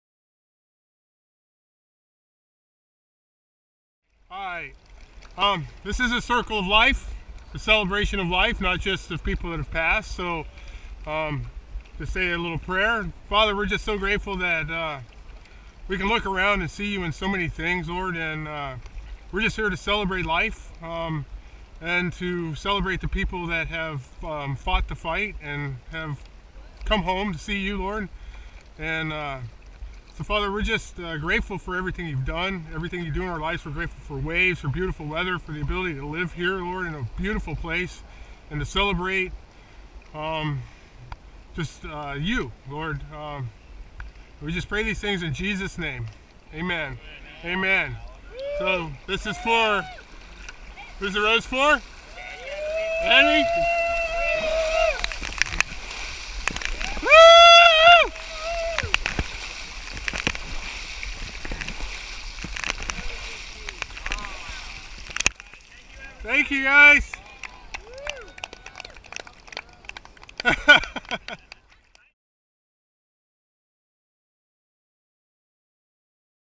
Cocoa Beach KIdney Walk the Circle of Life
NKFF Kidney Walk Faith, Prayer and Hope. The Circle of Life is Eternal.